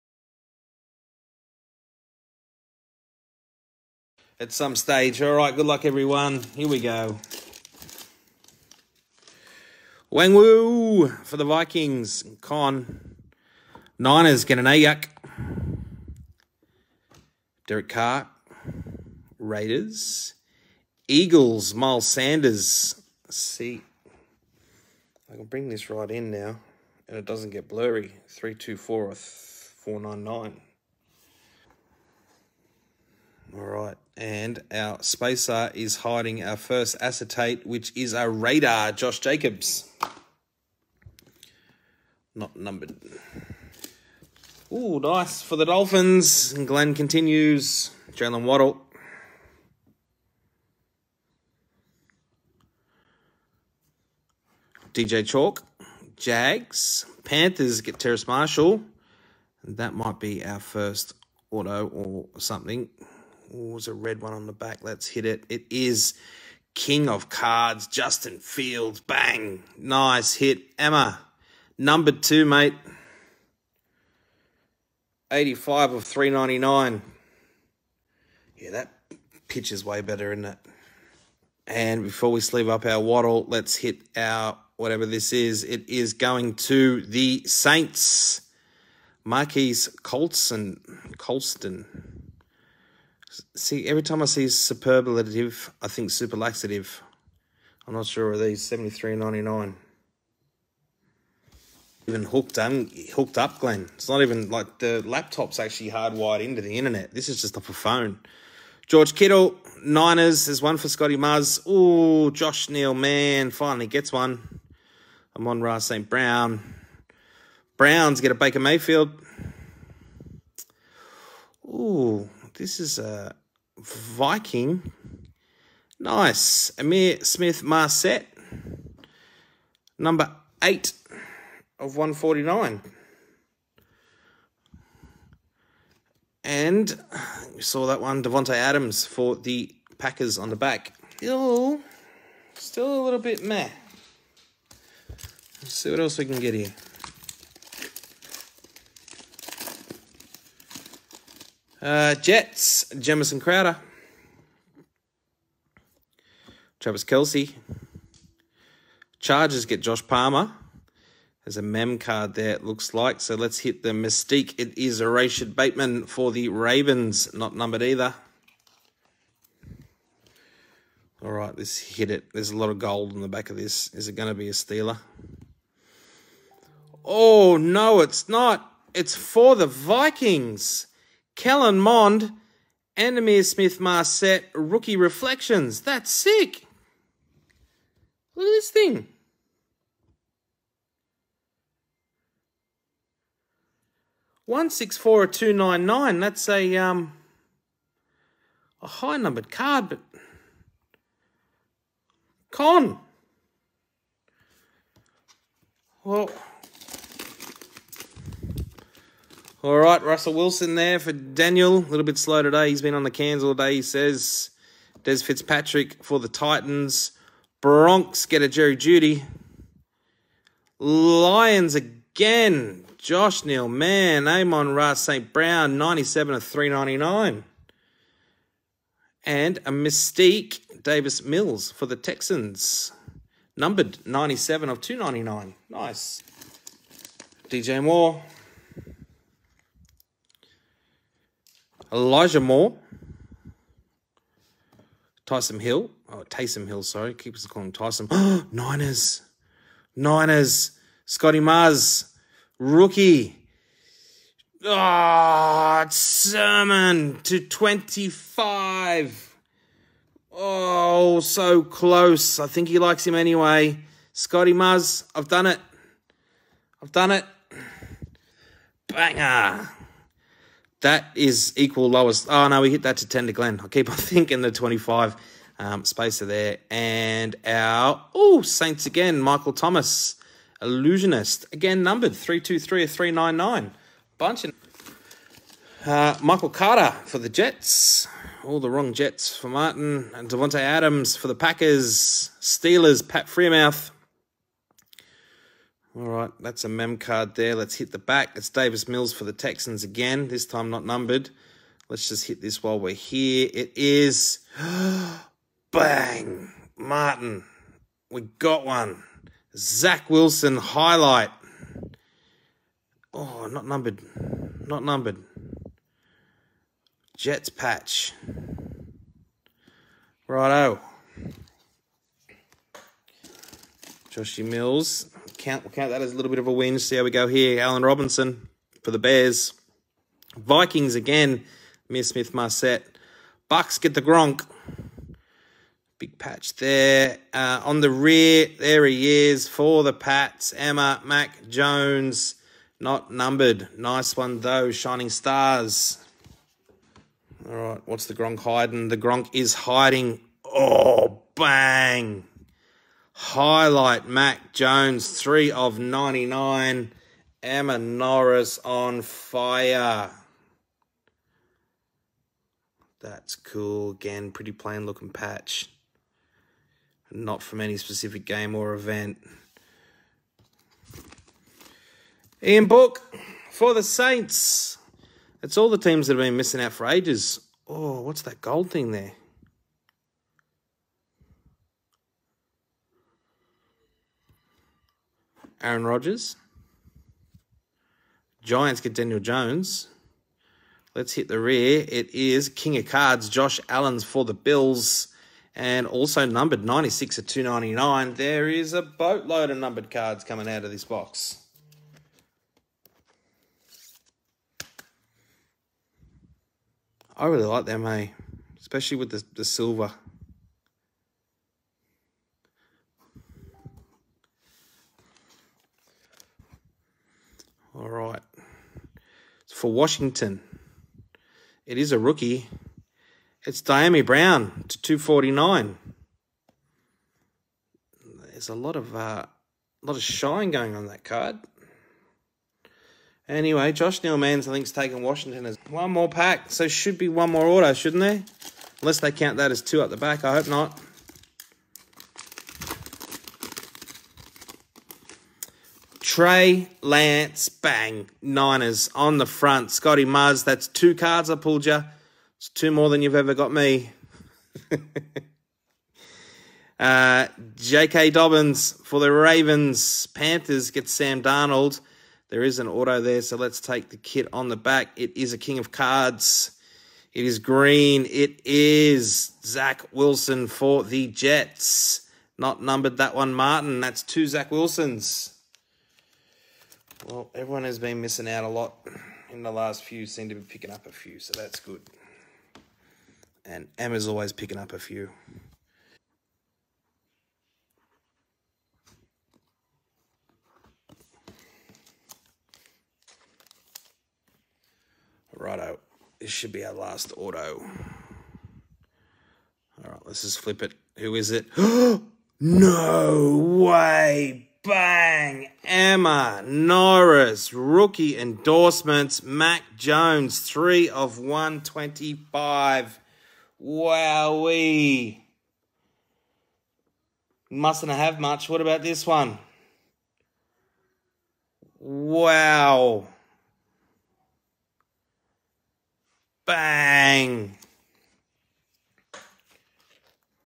Panini Illusions Football Hobby - Mac Jones Auto and a bunch of numbered cards
Check out break 455 Illusions hobby where we pull a monster Mac Jones Auto